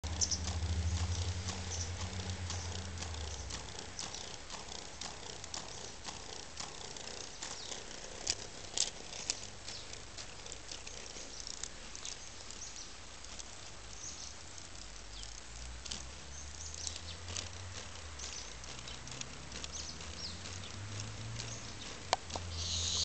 Anotogaster sieboldii - paret ova
Anotogaster sieboldii (Selys,1854); paret ova 15.viii.2006 Hanava-mathi Fucusima Japonia.